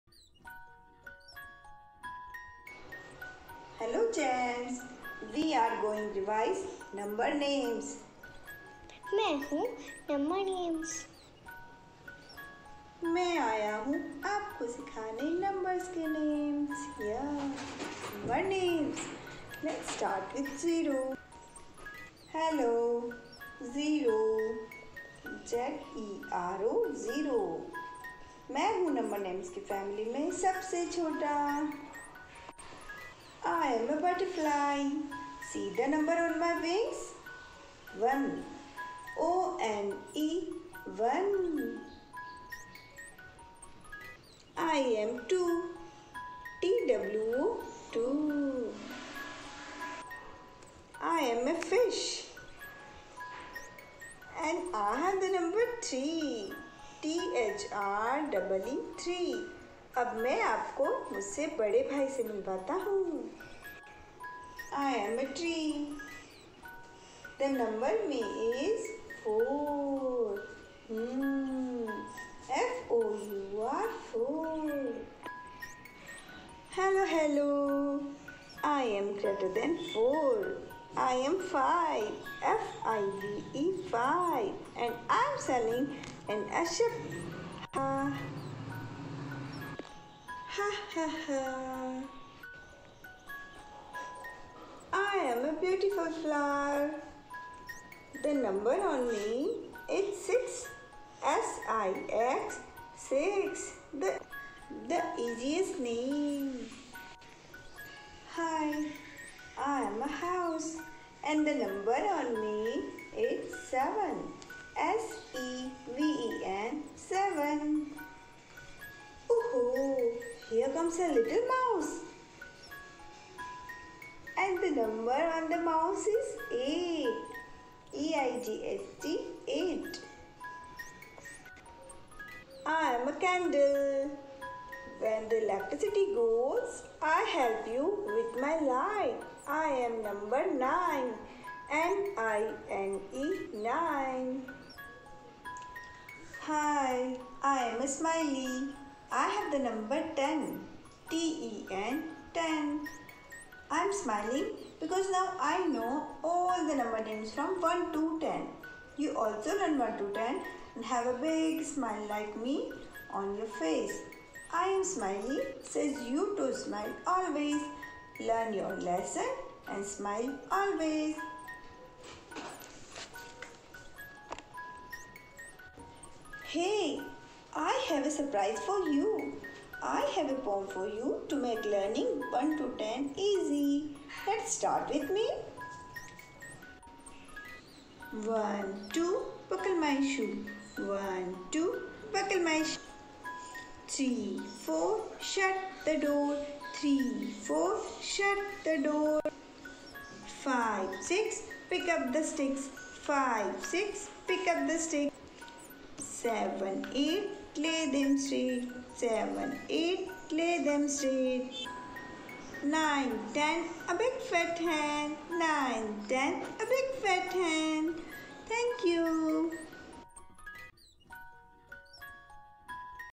Number names 1 to 10/ 1 to 10 poem by Adhyayan School Jodhpur
Welcome to Bag-less School !
No Home work ! No Burden !
A help to working parents.
Adhyayan School, Jodhpur, Rajasthan, INDIA

This video is for kids to teach them number names 1 to 10.